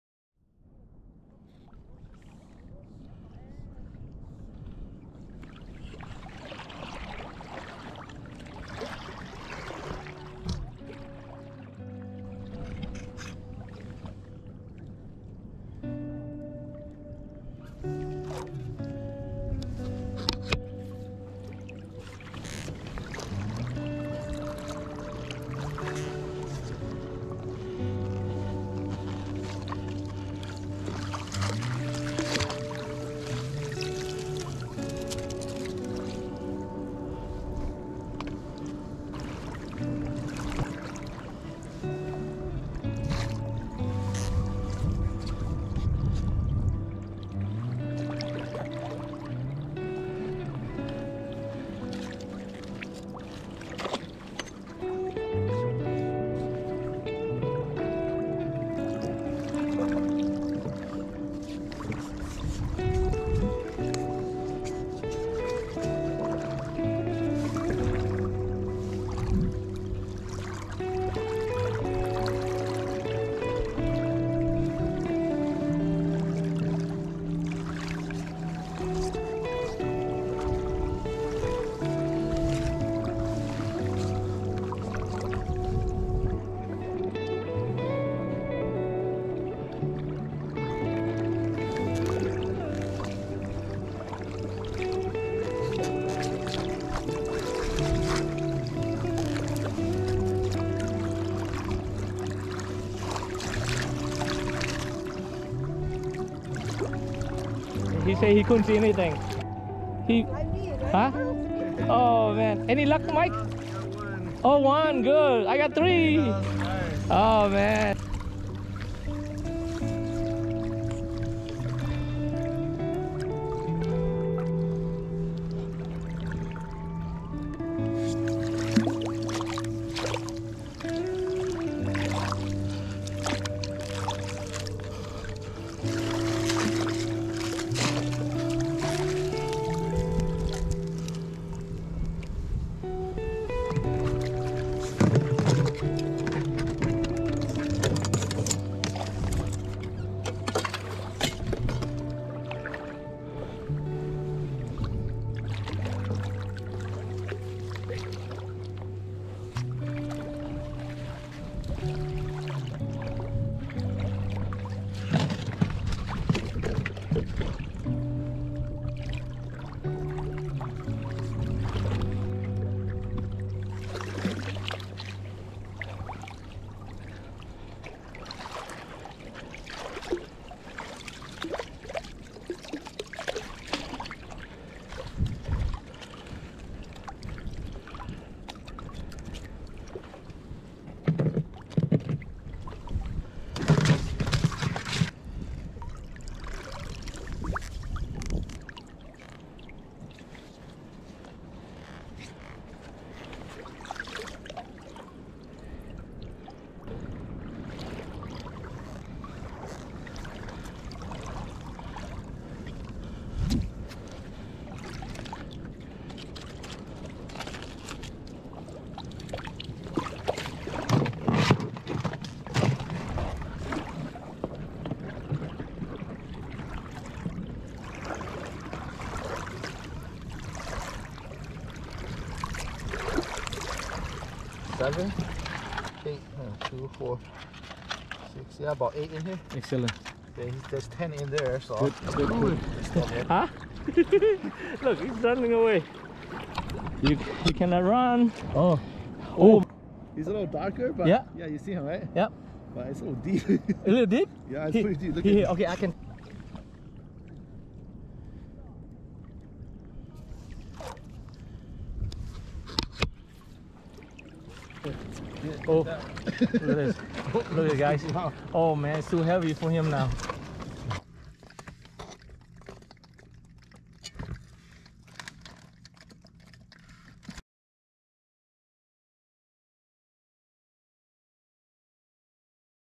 Vớt cua xanh Dungeness ban đêm
Một đêm vớt cua thành công mỹ mãn. Tất cả sáu người đều được giới hạn số cua xanh, mổi người mười hai con cua, tổng cộng là bảy mươi hai con!